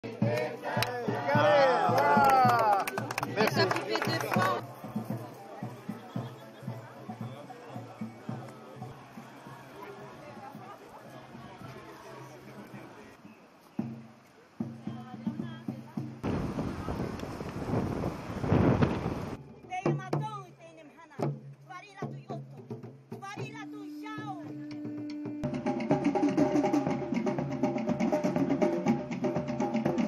Annick Girardin inaugure l'aménagement de la Pointe Vénus - 24/01/2018
La ministre des Outre-mer, Annick Girardin, accompagnée de toutes les autorités du Pays, a inaugurée mercredi 24 janvier 2018 le nouvel aménagement de la pointe Vénus, à Mahina.